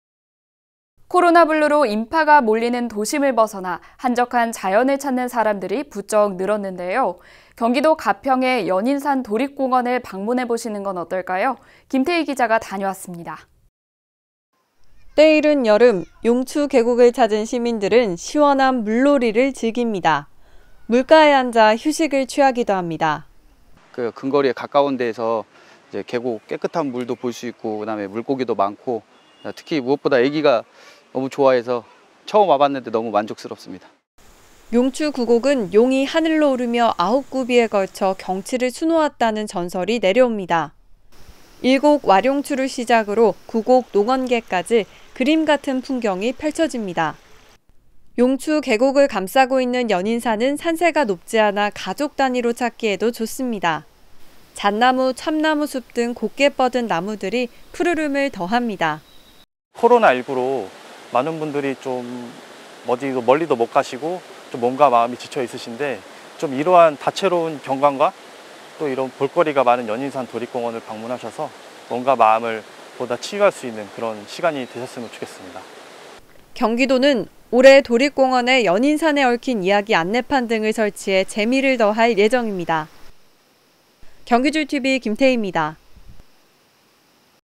연인산도립공원에서 시원하게 자연을 즐겨요
[앵커멘트]

코로나 블루로 인파가 몰리는 도심을 벗어나 한적한 자연을 찾는 사람들이 부쩍 늘었는데요.

경기도 가평에 연인산 도립공원을 방문해보시는 건 어떨까요?

김태희 기자가 다녀왔습니다.

[리포트]

1. [자막] 연인산도립공원/가평군 (6월16일)
때 이른 여름, 용추 계곡을 찾은 시민들은 시원한 물놀이를 즐깁니다.

물가에 앉아 휴식을 취하기도 합니다.

2. [자막] 김석주, 김회인, 김시윤/안산시 대부동
“근거리 가까운 데서 깨끗한 물도 볼 수 있고 물고기도 많고 무엇보다 아기가 좋아해서 처음 와봤는데 너무 만족스럽습니다.”

용추구곡은 용이 하늘로 오르며 아홉 굽이에 걸쳐 경치를 수놓은 전설이 내려옵니다. 1곡 와룡추를 시작으로9곡 농원계까지 그림 같은 풍경이 펼쳐집니다.

용추계곡을 감싸고 있는 연인산은 산세가 높지 않아 가족단위로 찾기에도 좋습니다.

잣나무, 참나무 숲 등 곧게 뻗은 나무들이 푸르름을 더합니다.

3. [자막] 이윤환 /경기도 공원녹지과 주무관
“코로나 19로 많은 분들이 멀리도 못 가시고 몸과 마음이 지쳐있으신데 다채로운 경관과 볼거리가 많은 연인산 도립공원을 방문하셔서 몸과 마음을 치유할 수 있는 시간이 되었으면 좋겠습니다.”

경기도는 올해 연인산에 얽힌 이야기 안내판 등을 설치해 재미를 더할 예정입니다.

영상 촬영, 영상 편집 : 김광석